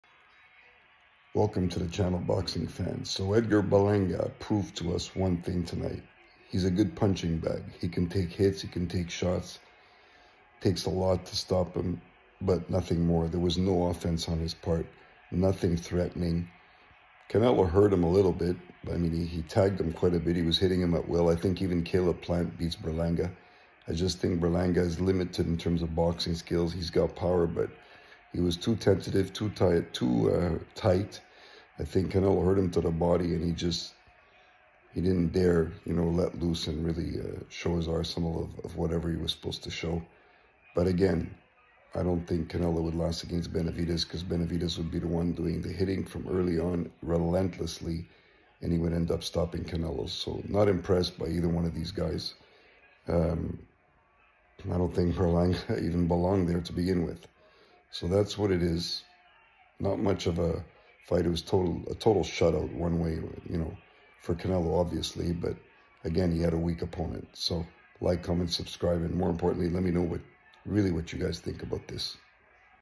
Edgar Berlanga nothing more than a sitting duck and a punching bag vs Ducker Canelo Alvarez .
A one way fight from start to finish as Edgar Berlanga  does nothing more than survive in a mismatch that should have never happened . David Benavidez knocks Canelo out if they meet up.